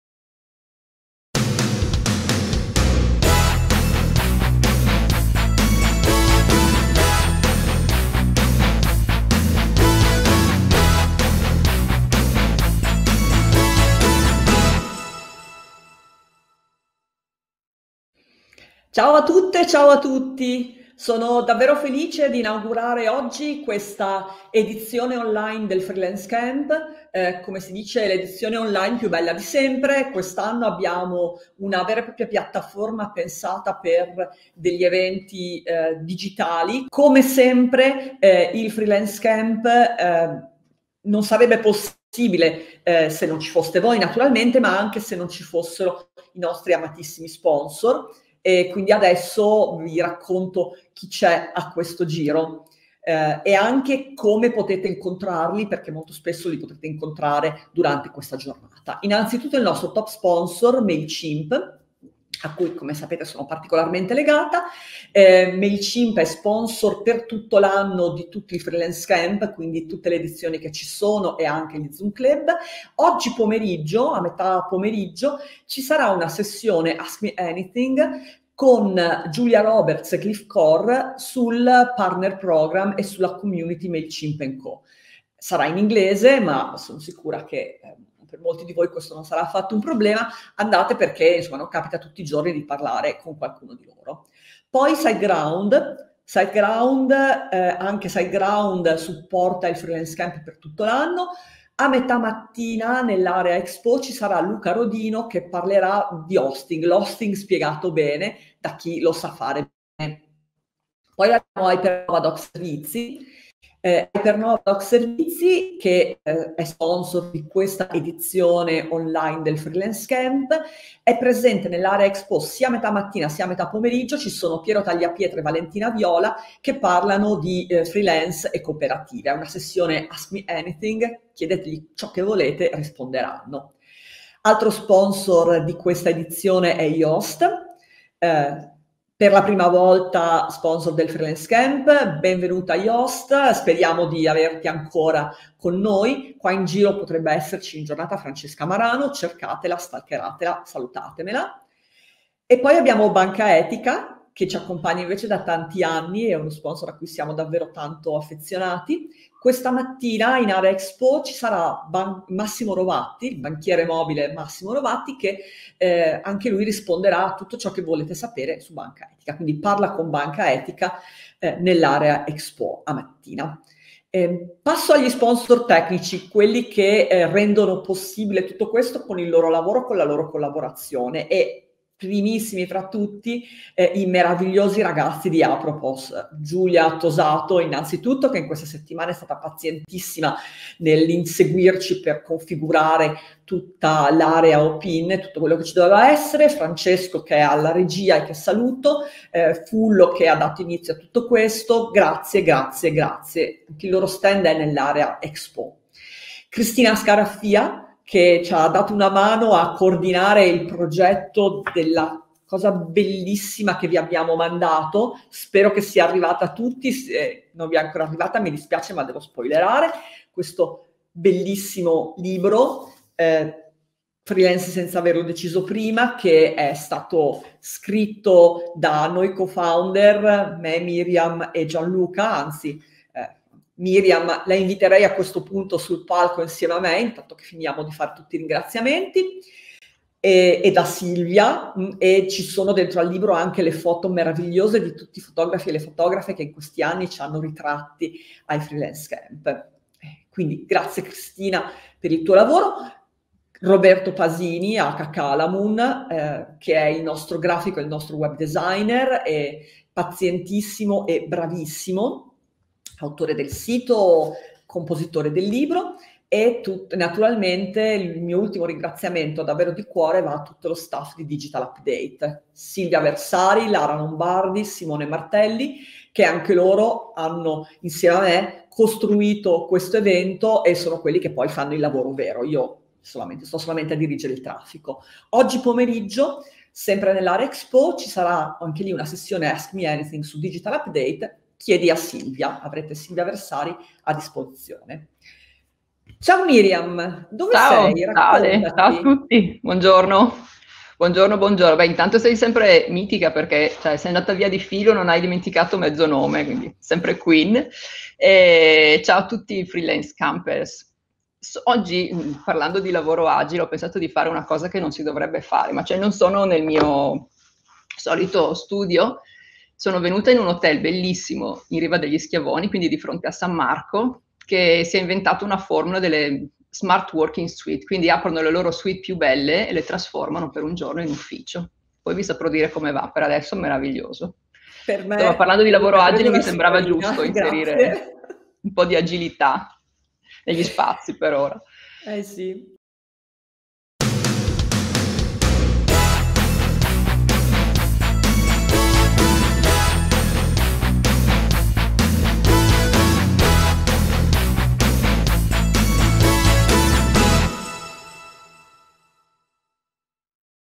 Inizia il Freelancecamp Online!
Alessandra Farabegoli e Miriam Bertoli aprono il Freelancecamp Online 2021.